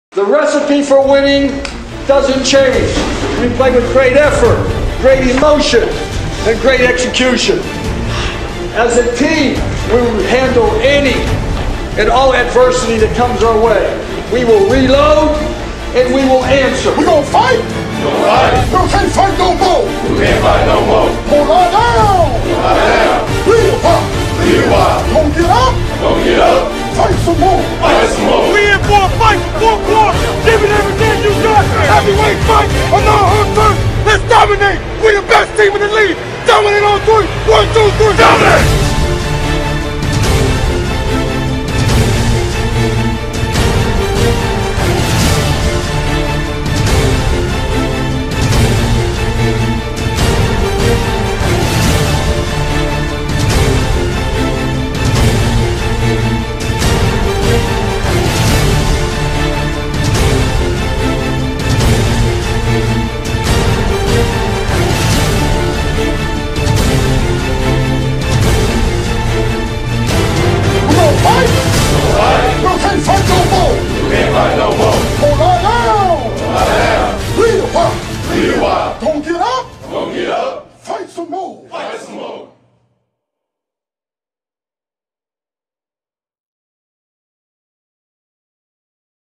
ACES Football Hype Film V.3
Footage from game vs. Ironton.

Coach: Scott Hinton